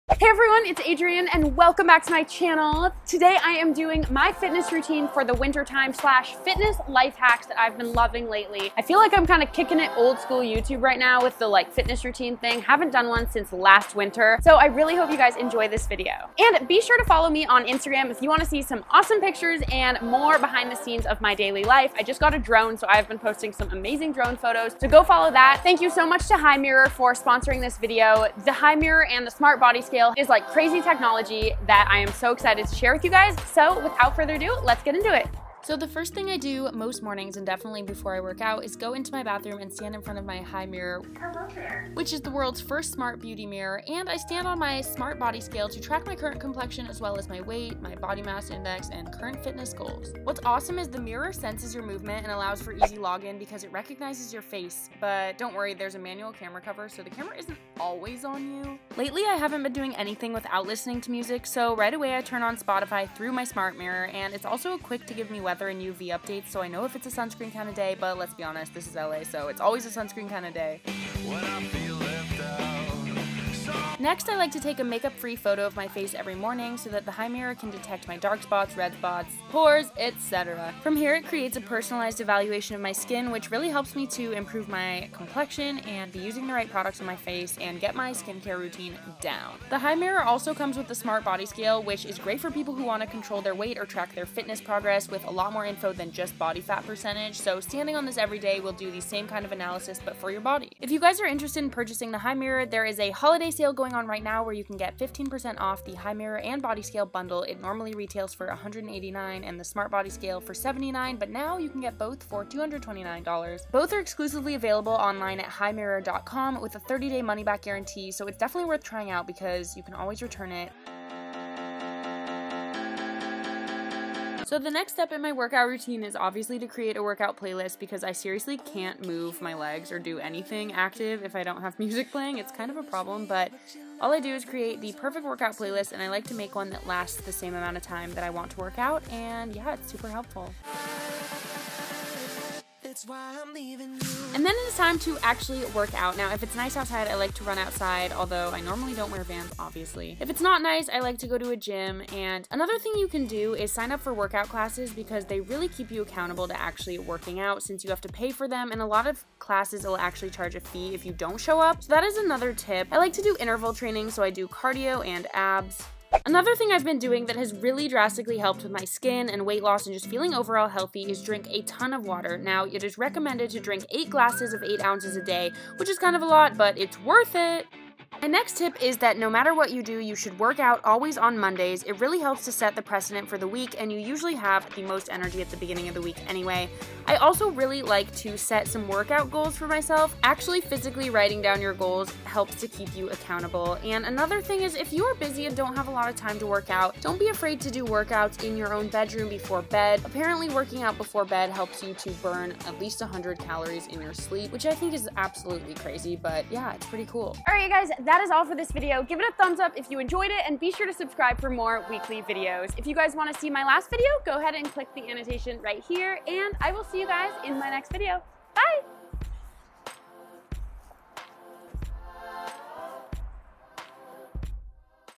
Winter Fitness Routine 2016 + Life Hacks!
Winner announced DEC. 15TH!
→ LET'S BE FRIENDS! ←

Hi guys! Today I'm showing you my updated fitness / workout routine for 2016 along with some personal fitness hacks that I use on the regular! I hope you enjoy and that it maybe gives you some motivation to get your fitness on this winter. These tips help me to stay motivated and happy! Love you so much!

For one month of FREE designer eyewear, use code FINCH